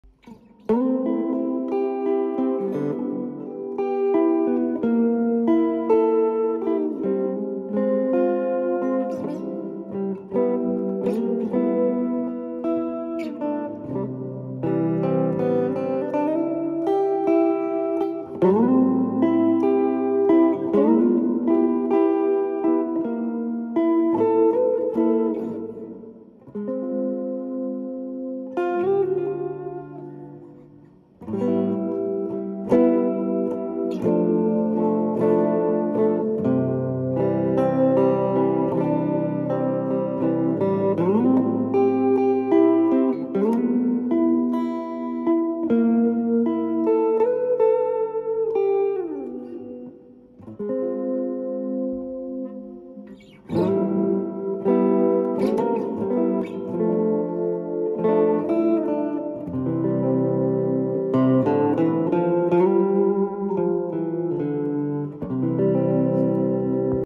Off the cuff doowop stuff (tele take1)
Yamaha Thr desktop amp
Amp reverb & chorus
MXR phase 90
Handmade pine body Tstyle with strat pickups

Original? Tune idea